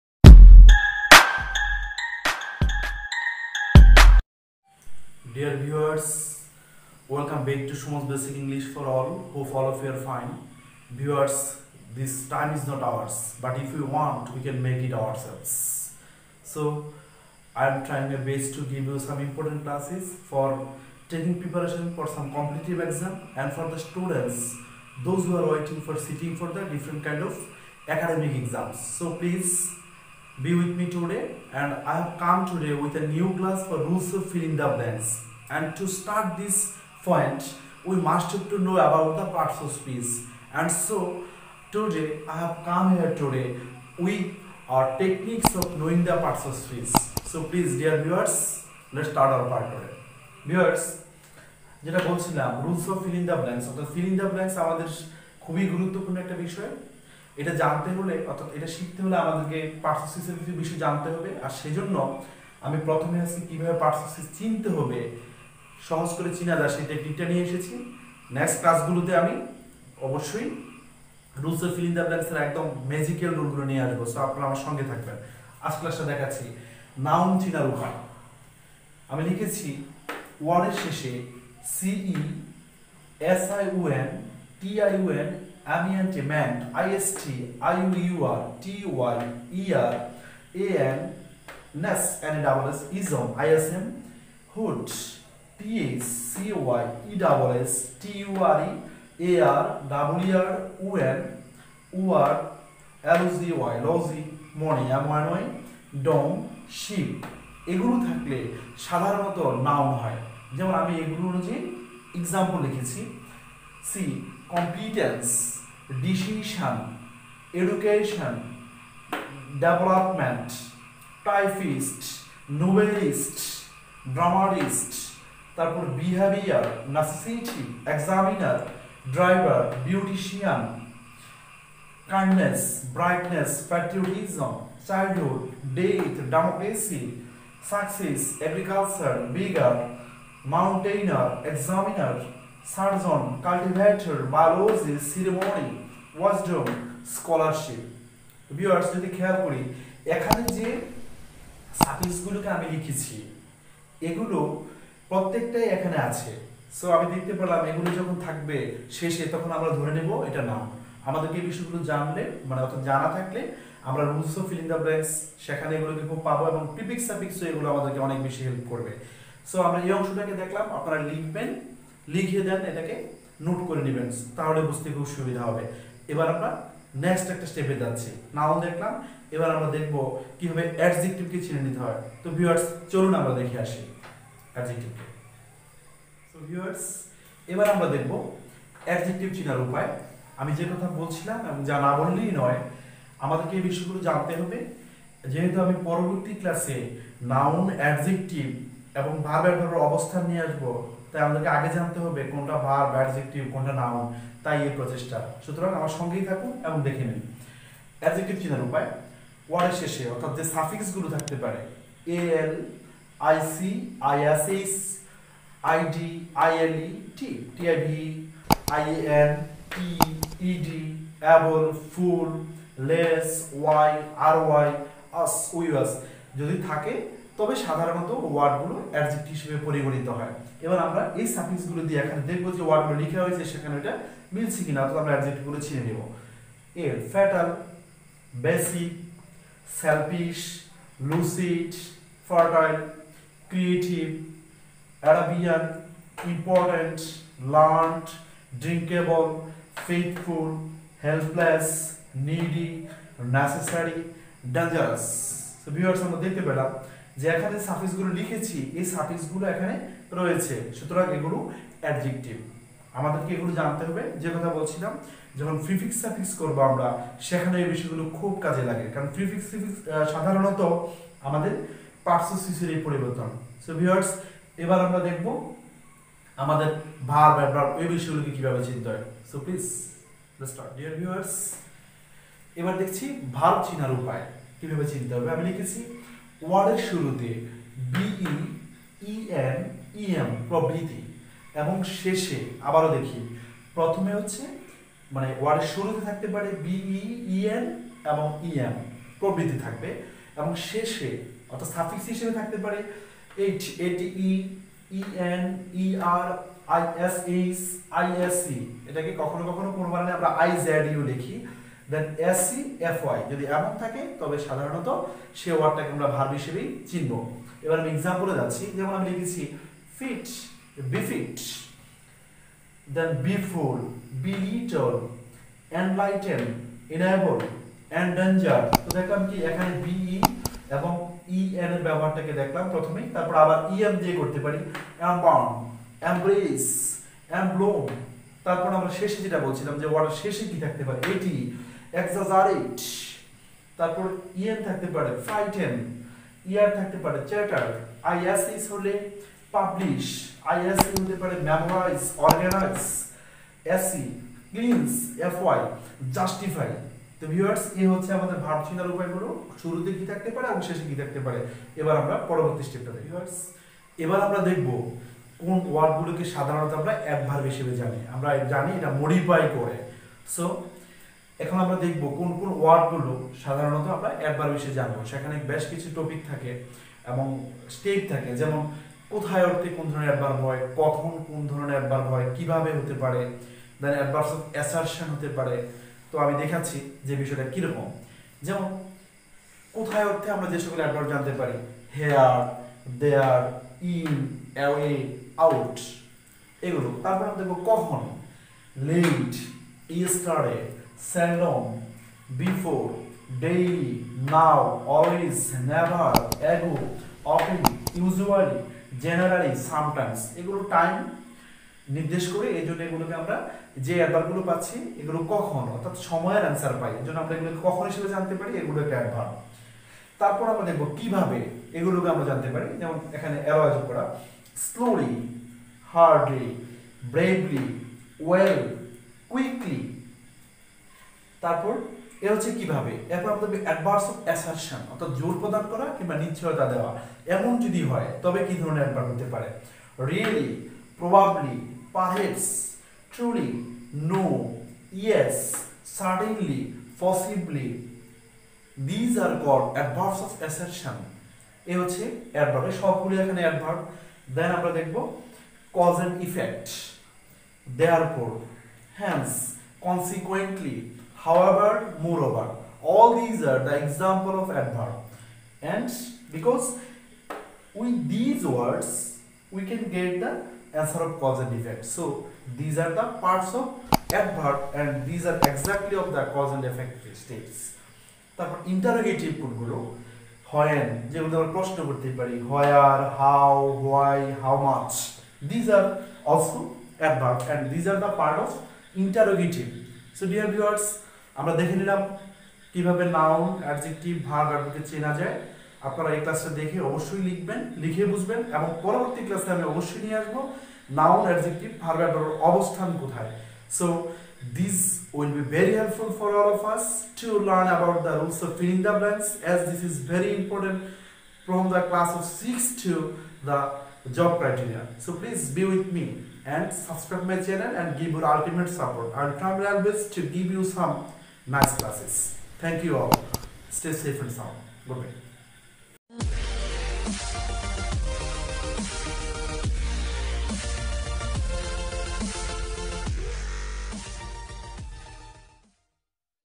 Parts of Speech. Short techniques.
The best way to fill the gaps.
Parts of Speech. Identification of Noun, Adjective, Verb and Adverb is very importont for knowing the rules of fill in the blanks. Here in this content I have shown some easy techniques for identifying different kinds of Parts of Speech. With this content all the viewers will be benefitted as it it wil help them to know the Parts of Speech.